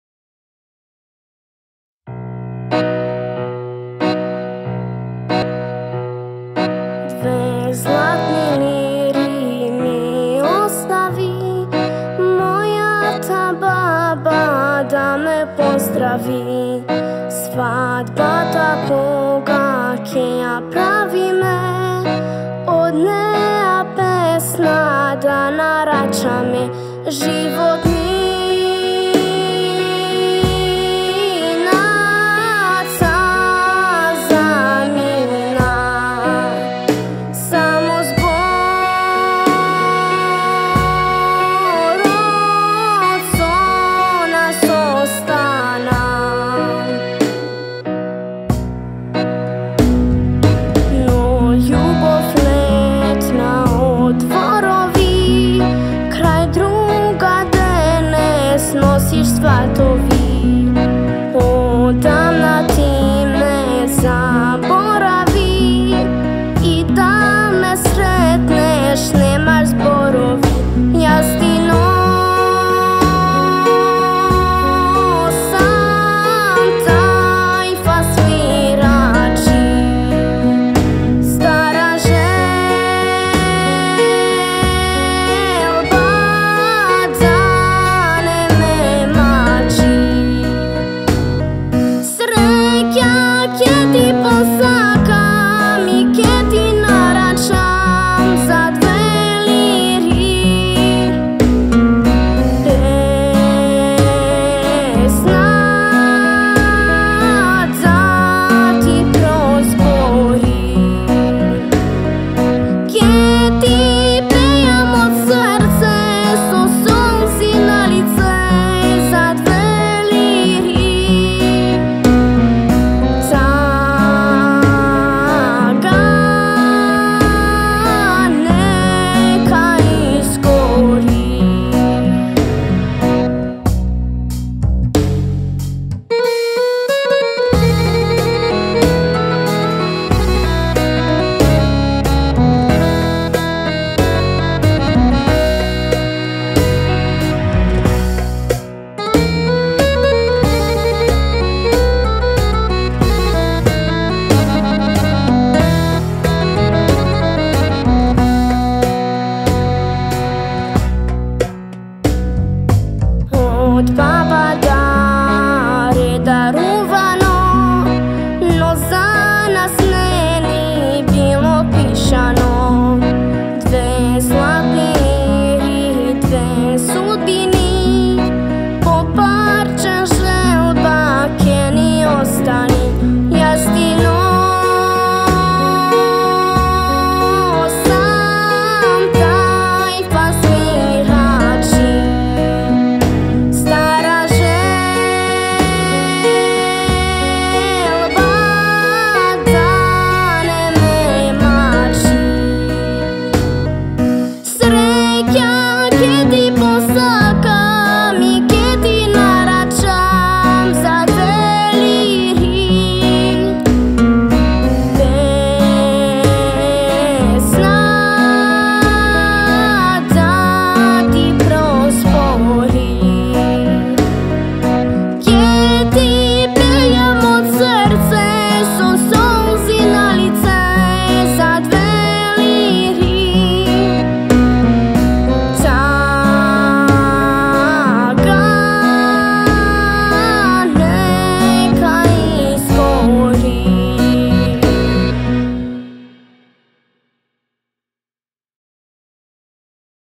Irena Mitеvska(Ти имаш талент)
Ирена Митевска е најмладиот вокал во екипата на „Ти имаш талент“,проект на Здружението за деца и млади „С.О.С Бубамра“-Каменица.

Аранжман: Маријан Стоименов Ѕика
Дизајн:Марија Витановска